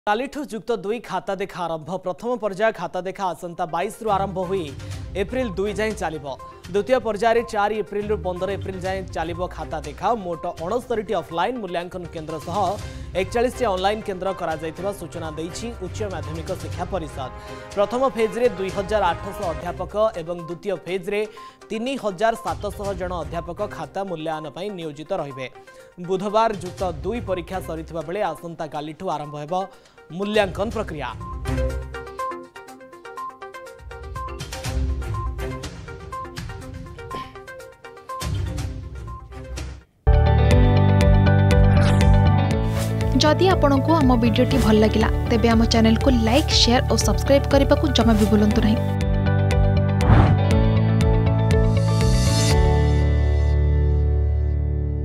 କାଲିଠୁ ଯୁକ୍ତ ଦୁଇ ଖାତା ଦେଖା || Plustwo exam
69ଟି ଅଫଲାଇନ ଓ 41ଟି ଅନଲାଇନ କେନ୍ଦ୍ରରେ ହେବ ଖାତା ଦେଖା । ପ୍ରଥମ ପର୍ଯ୍ୟାୟରେ ୨ହଜାର ୮୦୦ ଓ ଦ୍ବିତୀୟ ପର୍ଯ୍ୟାୟରେ ୩ହଜାର ୭୦୦ ଅଧ୍ୟାପକ  ମୂଲ୍ୟାୟନରେ ହେବେ ନିୟୋଜିତ ।